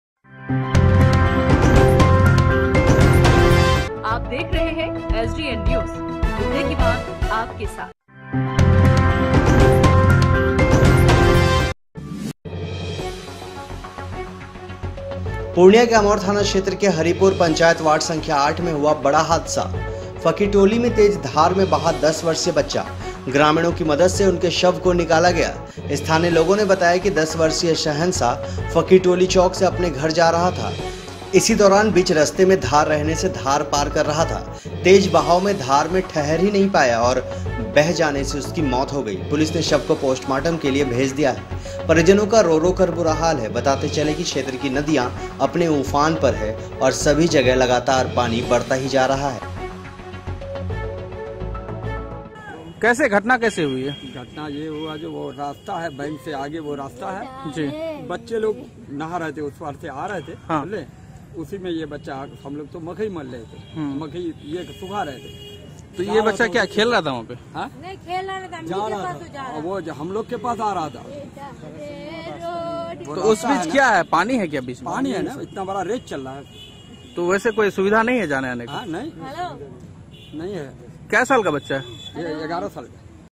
नदियों का कहर - तेज धार में बहा 10 वर्षीय बच्चा - ग्रामीणों की मदद से उसके शव को निकाला गया -
पुर्णिया के अमौर थाना क्षेत्र के हरीपुर  पंचायत वार्ड नंबर 8 में हुआ बड़ा हादसा । फकीर टोली में तेज धार में बहा 10 वर्षीय बच्चा । ग्रामीणों की मदद से उसके शव को निकाला गया । स्थानीय लोगों ने बताया कि 10 वर्षीय शहंशाह फ़क़ीर  टोली चौक से अपने घर जा रहा था , इसी दौरान बीच रस्ते में धार रहने से धार पार कर रहा था । तेज बहाव में धार में ठहर नहीं पाया , बह जाने से उसकी मौत हो गई । पुलिस ने शव को पोस्टमार्टम के लिए भेज दिया है । परिजनों का रो रो कर बुरा हाल है । बताते चलें की क्षेत्र की नदियां उफान पर रहने से सभी जगह पानी भर गया ।